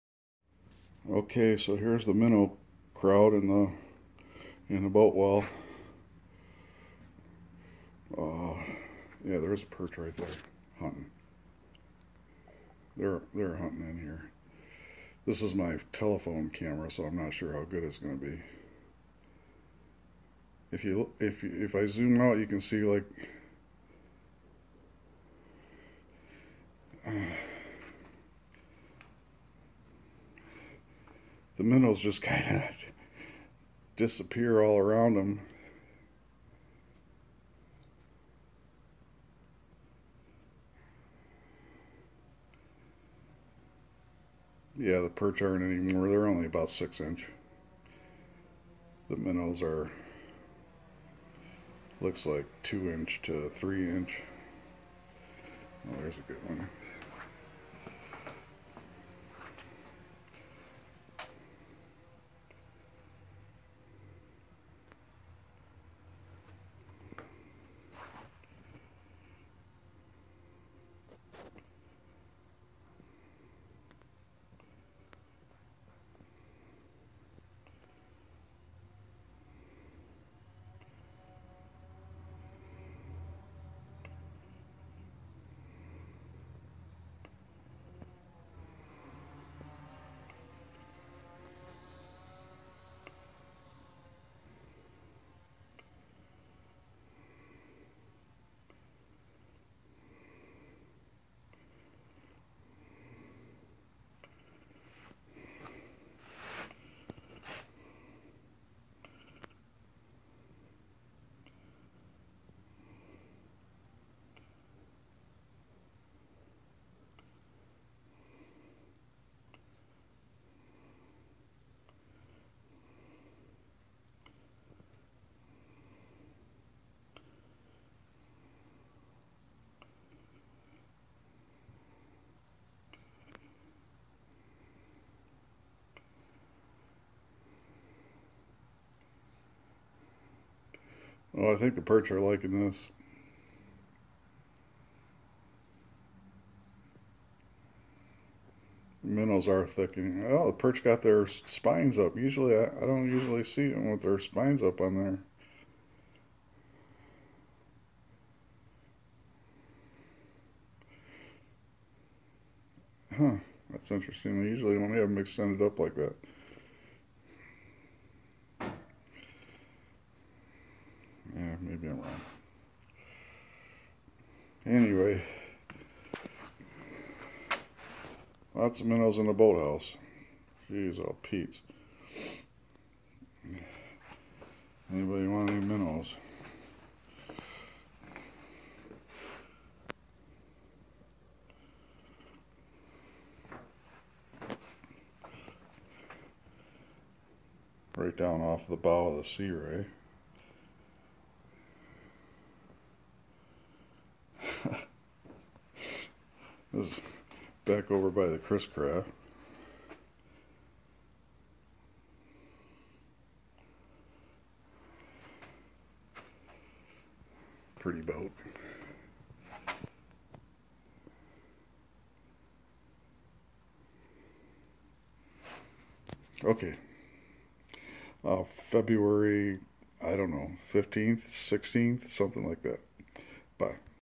Perch vs Minnows
Not real exciting but it is all in the boat well.